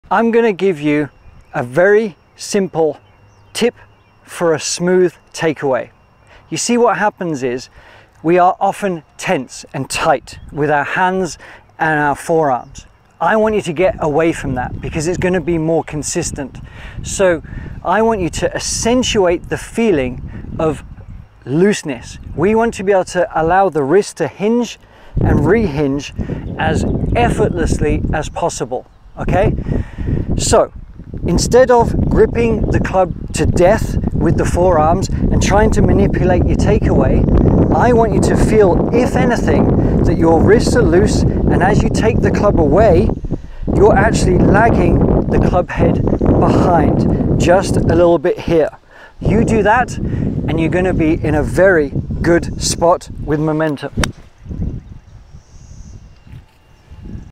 How To Start A Golf Swing For Effortless Power And Consistency | The Ground And Soft Wrist Hinge
How to start a golf swing and how to use the wrists correctly in the golf swing. It this golf lesson, Alex Fortey from the Art of Simple Golf gives you a simple golf tip to improve your golf swing takeaway and wrist hinge that is more consistent and has more flow and tempo. More flow means more reliability and an effortless power golf swing not just on the takeaway but the backswing and golf swing release that is easier especially for beginner golfers and senior golfers too.

So if you want to know how to start a golf swing then use the soft wrists feel and use momentum from your feet and use the ground to propel the start of the golf swing.

Enjoy this golf video to help you get closer to having a perfect golf swing.. YOUR perfect golf swing